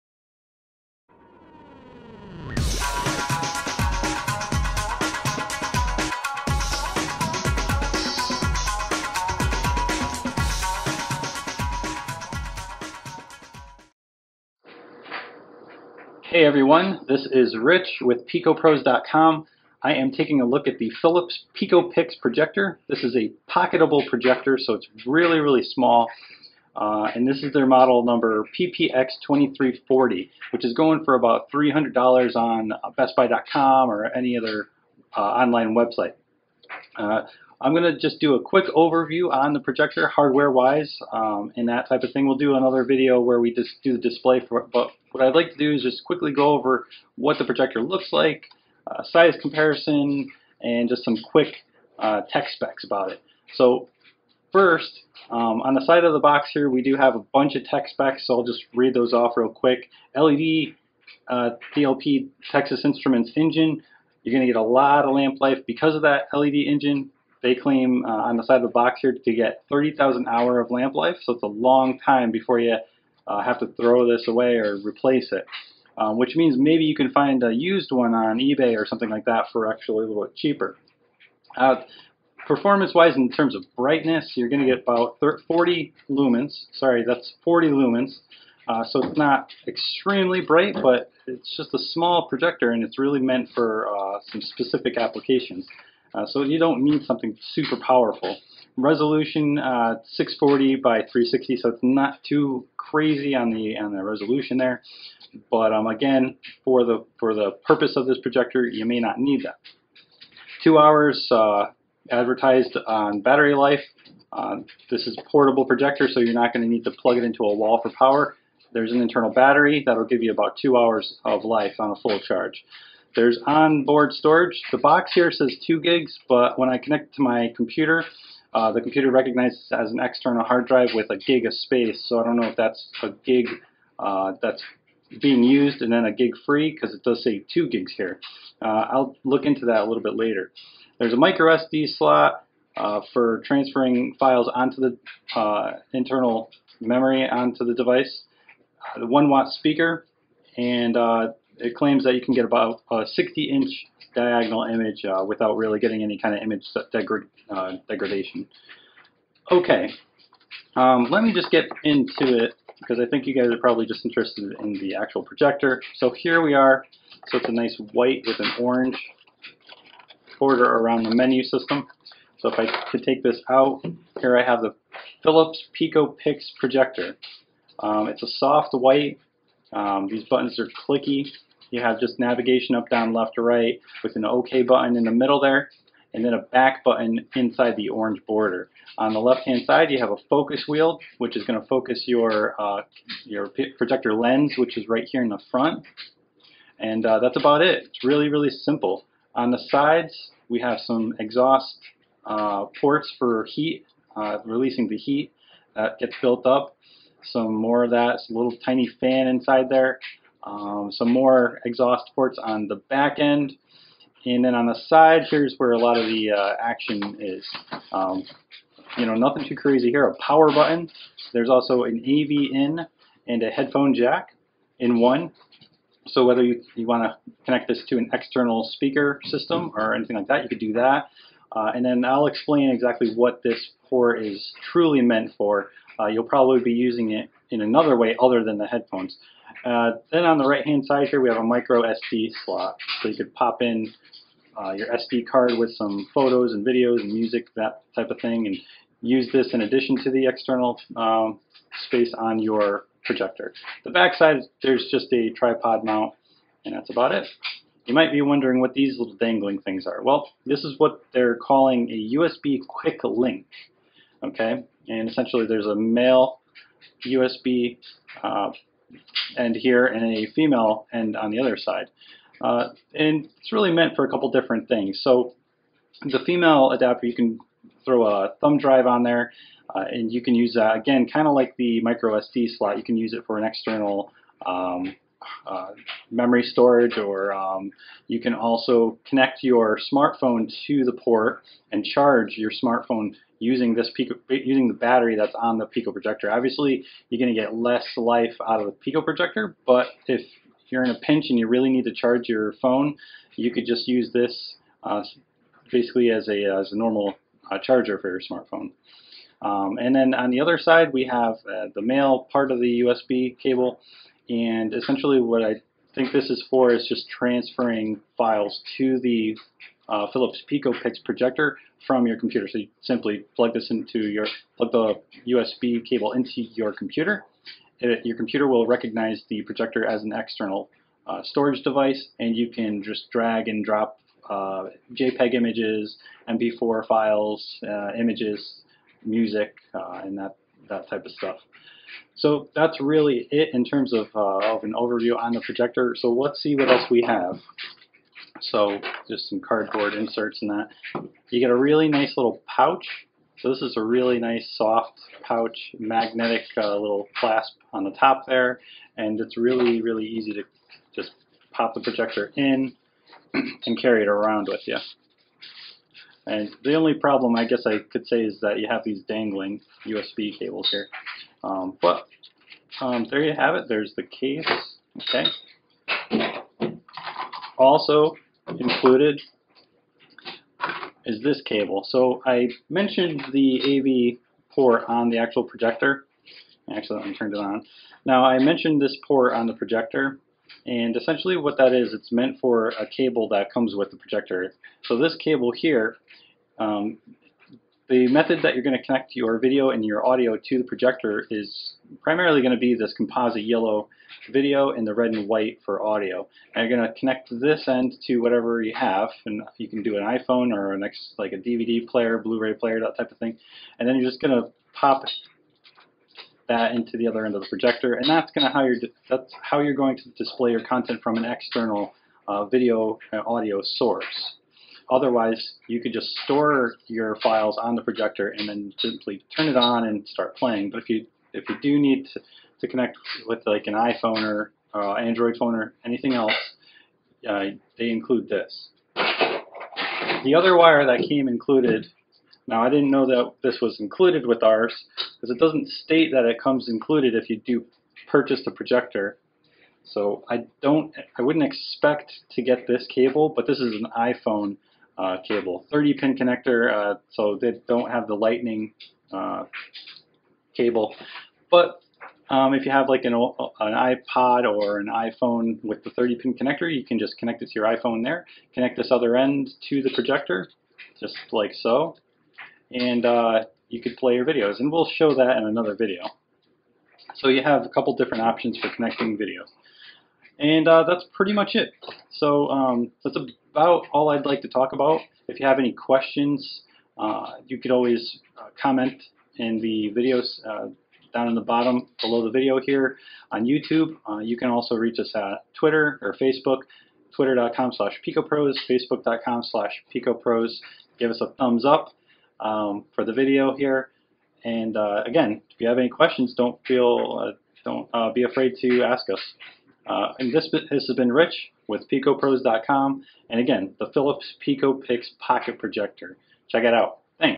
Philips PicoPix Intro
We unbox the Philips PicoPic pocket projector and show off the hardware. Compact and simple to use, this is the perfect pico projector for carrying with you on day trips, etc. Powerful for most settings, you'll get long lamp life with the 40 lumen LED projector display by Texas Instruments.

In terms of connectivity, it shines at displaying video, music, and photo content from the internal 2GB storage. Simply transfer files onto the Philips PicoPix from most laptops, and they are ready whenever you need them. Connecting to an external media source, such as a laptop/smartphone/etc is also easy, although it does require a cable to be carried along with the media player and projector. You'll primarily use composite (yellow) video, so check to be sure you're devices allow for that type of video output.

Quick Specs:
- 40 lumen, LED projector engine
- 60" diagonal images
- 640x360 pixel resolution
- 2 hour life on a full charge, with rechargeable internal battery
- micro SD slot and USB port for thumb drive, to expand storage
- lightweight and small
     - 4.6 oz / 130g
     - 3.8x2.1x.07 inches / 96.7x54x22 mm